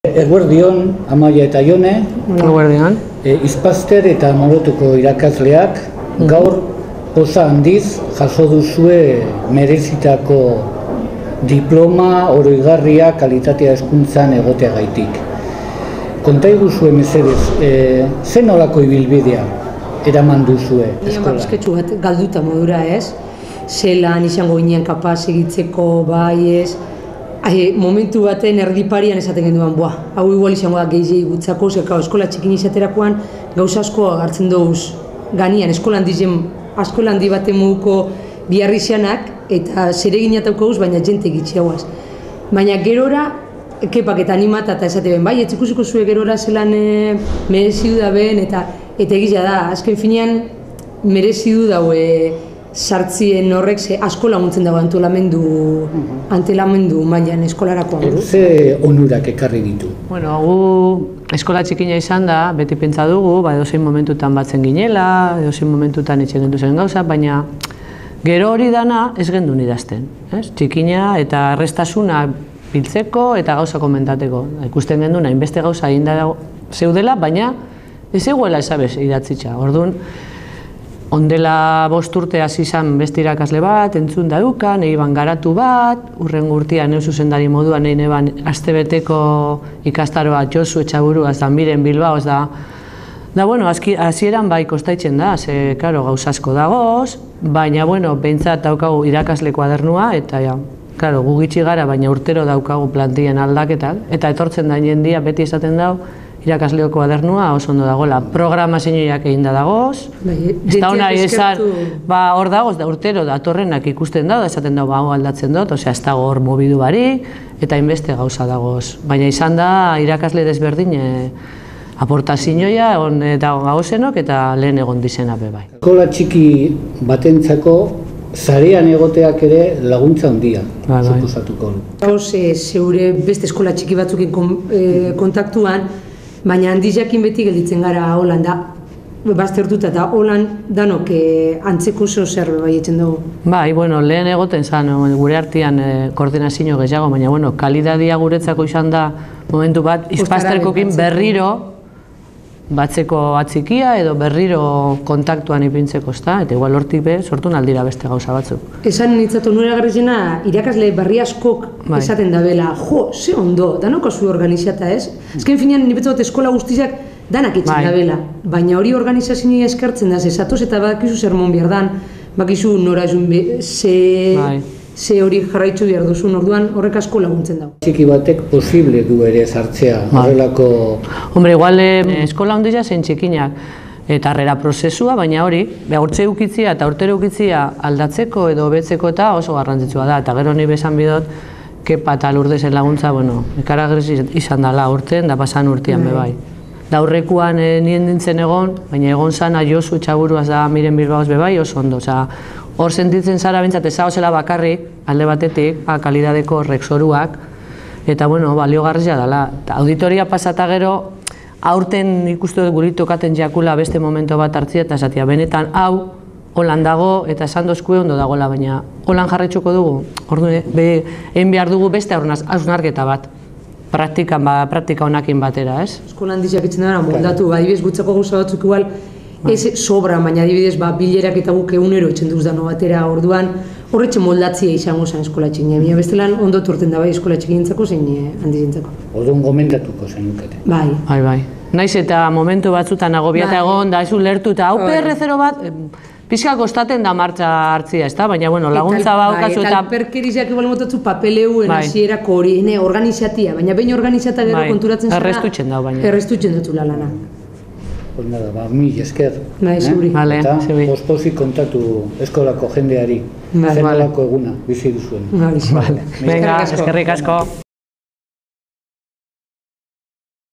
ispaster_amoroto
ispasterreko Amaia Zabala eta Amorotoko Jone Urraza BNko Jon Lopezekin solasean. Berritzegune Nagusiak antolatutako Kalitatea Hezkuntzan jardunaldietan